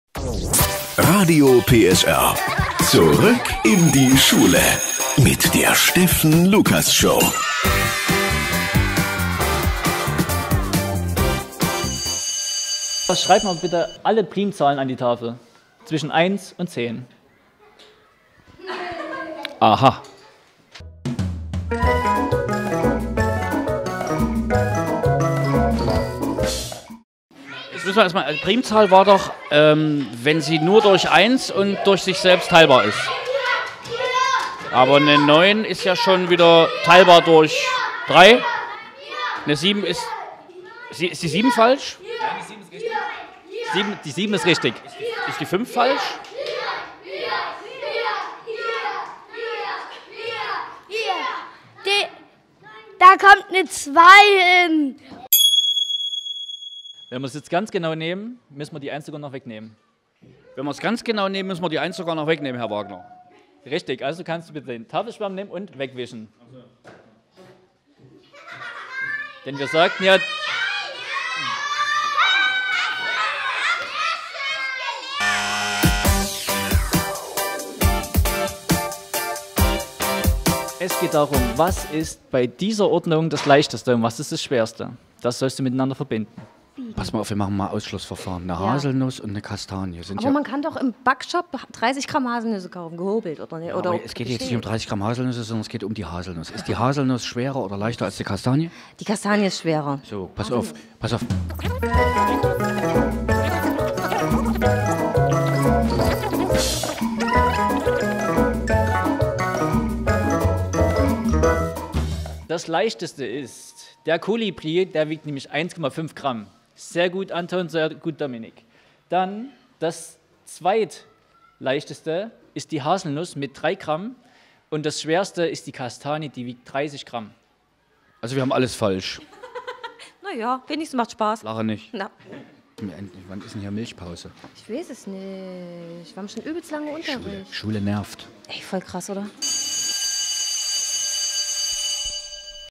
Zurück in die Schule - Mathe
Claudia Switala und Steffen Lukas gehen nochmal zur Grundschule!
Heute: Mathe!